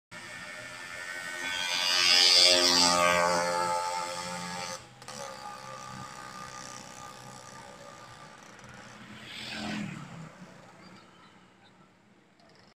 2 stroke motorcycle
The sound of annoying noisy 2-stroke engine motorcycle with sidecar, stands out among other vehicles.
Shot in front of Ynares Gym gate in Antipolo City, Rizal